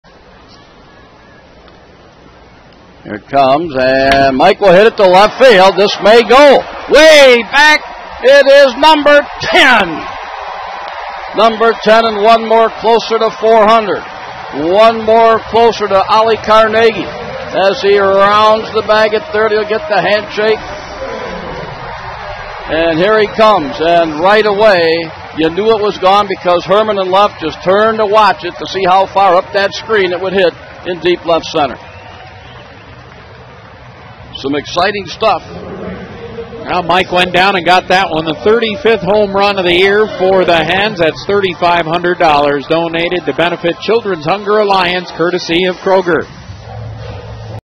Mud Hens' Hessman clubs homer No. 399
Mud Hens' Mike Hessman slugs the 399th homer of his MiLB career as Toledo takes on Rochester